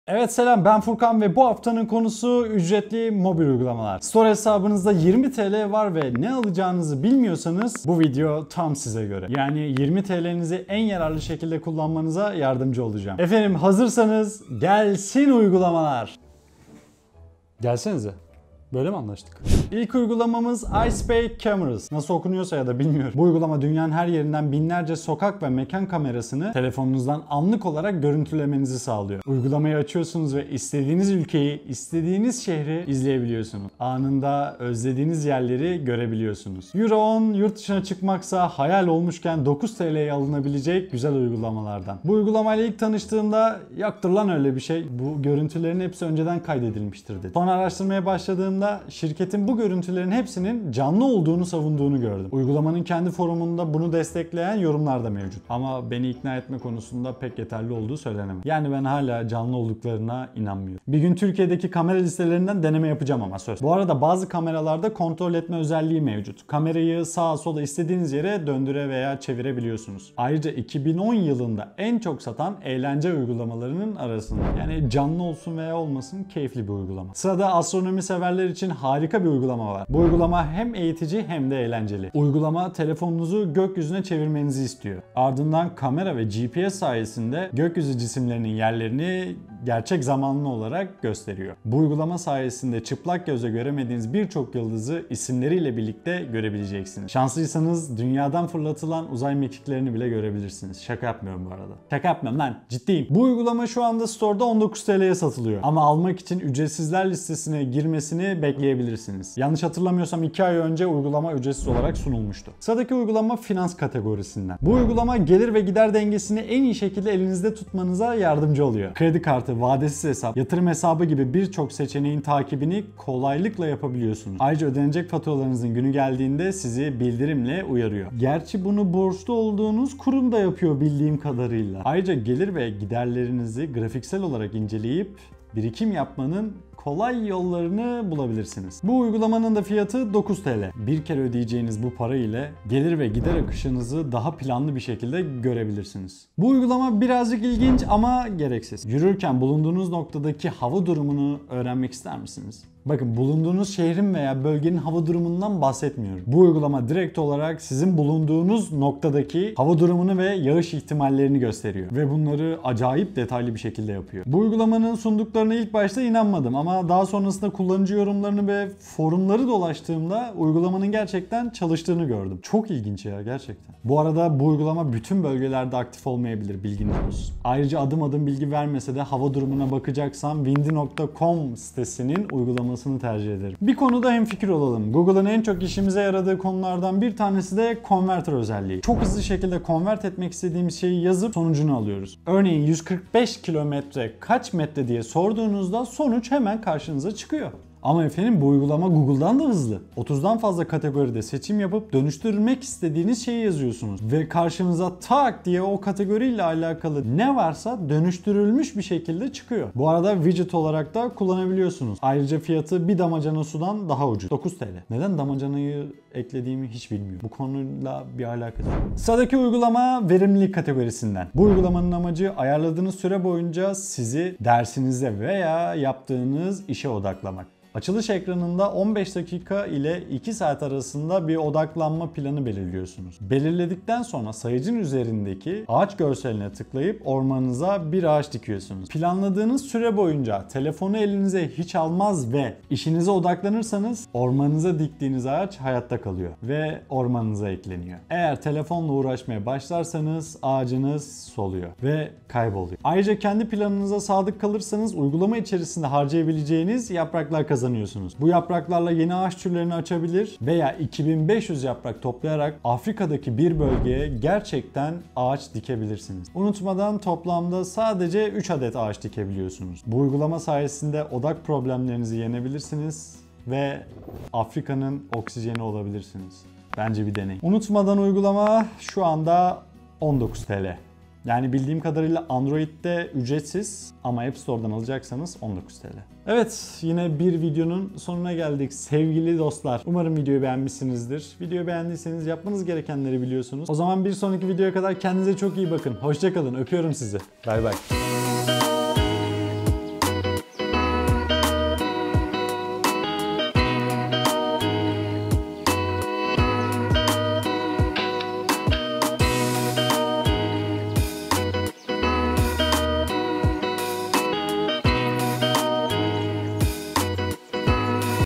TEK TUŞLA SOKAK KAMERALARINA BAĞLAN! (En iyi mobil uygulamalar )
TEK TUŞLA SOKAK KAMERALARINI İZLE!

Evet,herkese selamlar bu videoda, 20 tl altı en iyi uygulamaları derledim ,aralarında günlük hayattaki işlerinizi kolaylaştıracak uygulamalarda mevcut.Bu arada ücret ödemek istemiyorsanız uygulamaların ücretsizler listesine girmesini bekleyebilirsiniz , Videoyu beğenmeyi ve abone olmayı unutmayın dostlar :)